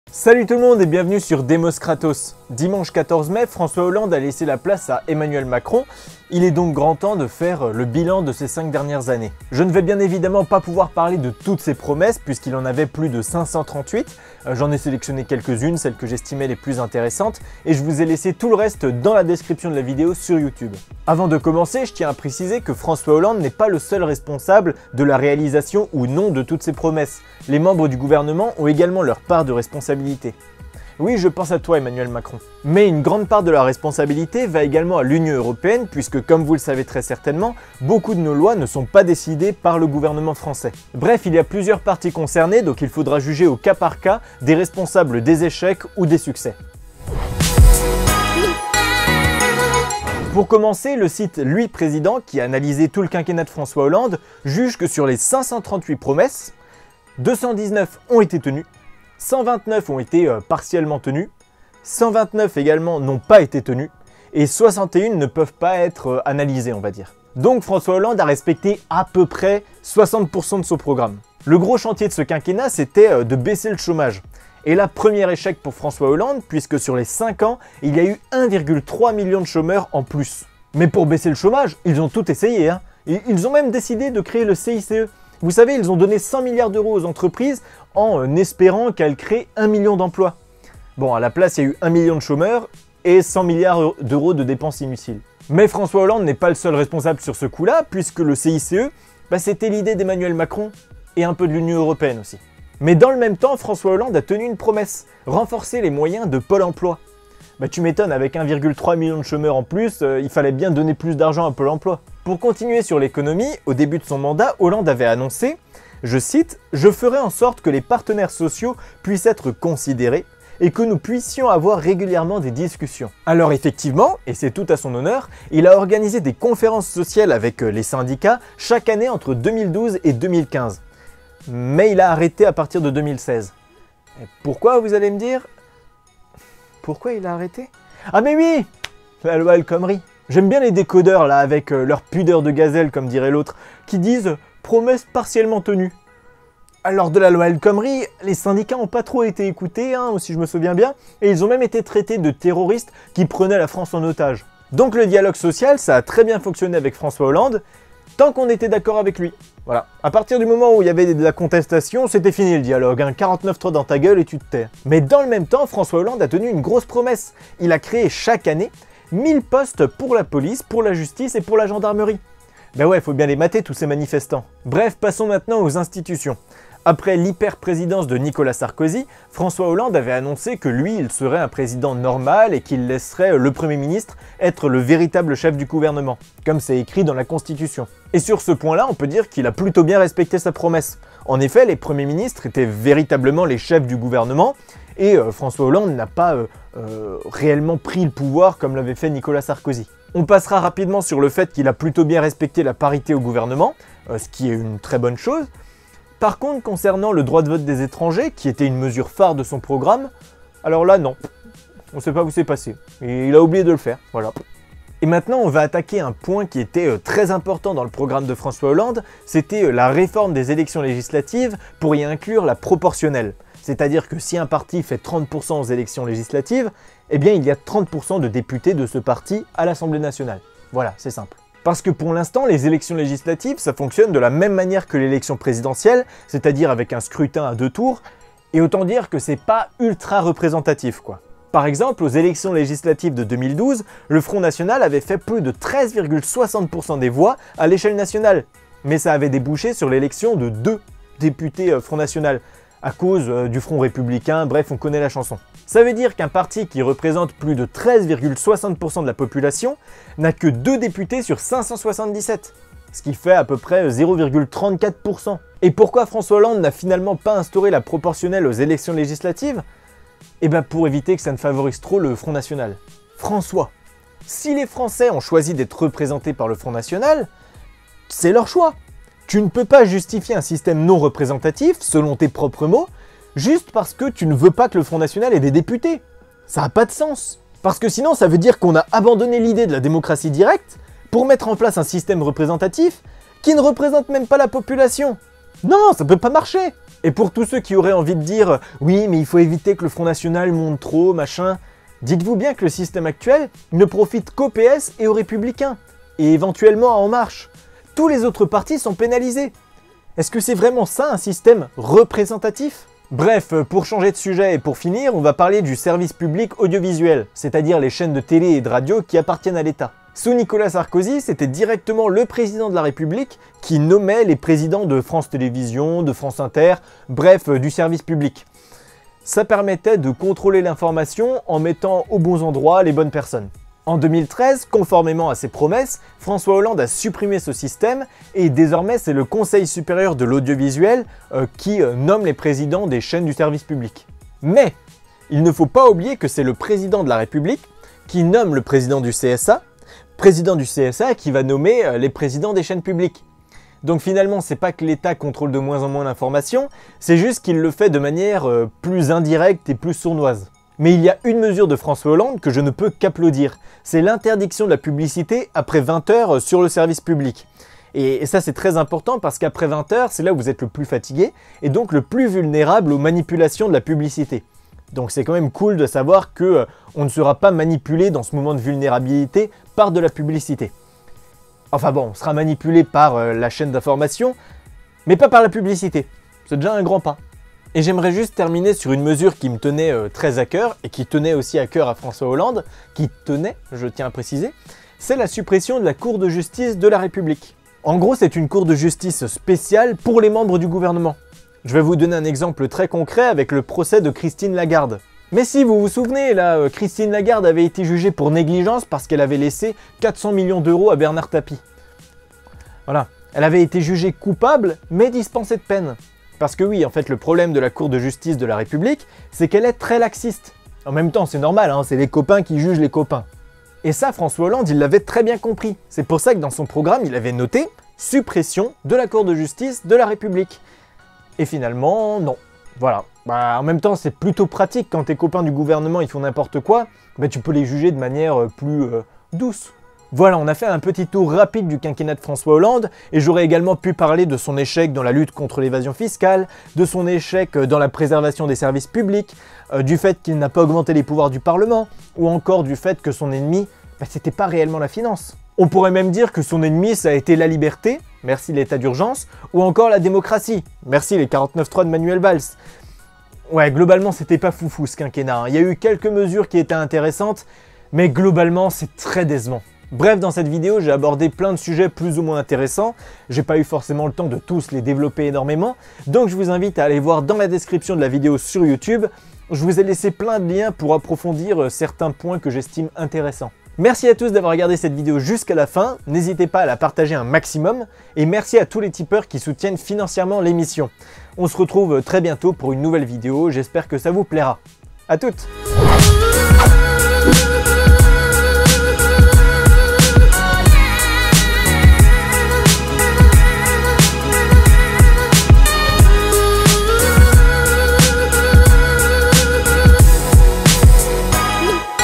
BILAN DU MANDAT DE FRANÇOIS HOLLANDE
C'est terminé, François Hollande a quitté l'Elysée. Bilan rapide de ce qu'il aura accompli durant son mandat.

18 380 (nombre d'abonné, aucun rapport avec l'énigme ^^)